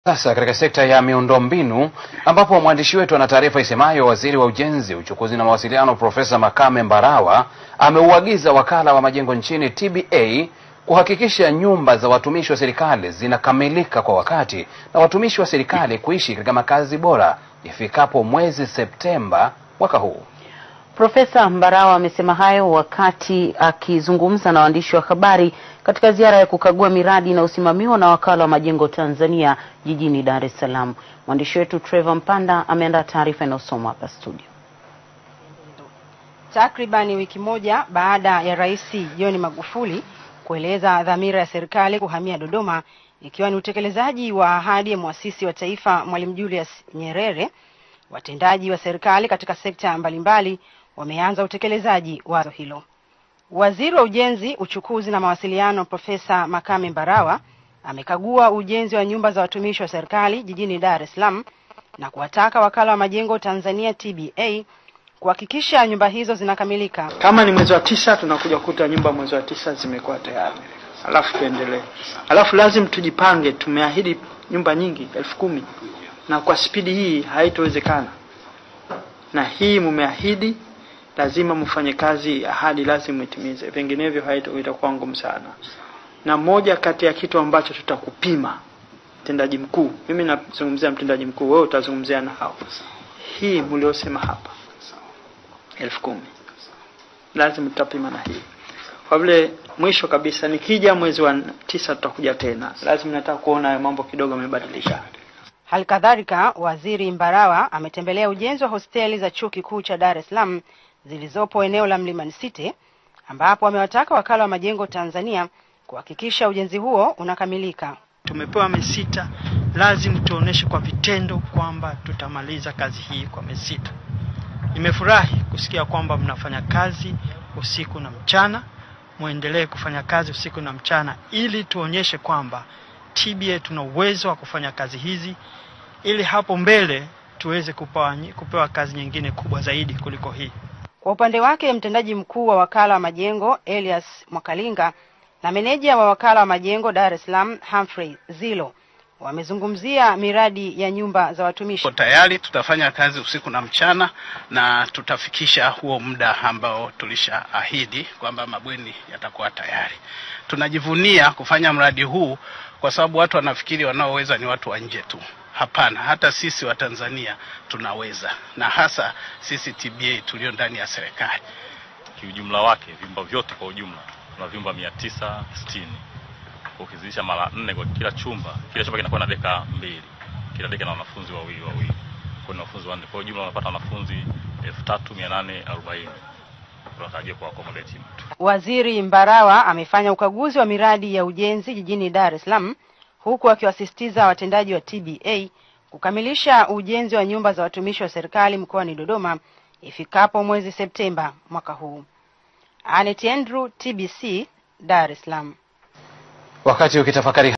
Ujenzi Nyumba za Watumishi Dar Es salaam
Waziri wa ujenzi, uchukuzi na mawasiliano Prof Makame Mbarawa, amemuagiza wakala wa majengo nchini TBA kuhakikisha anakamilisha majengo yote ya watumishi wa serikali ifikapo Septemba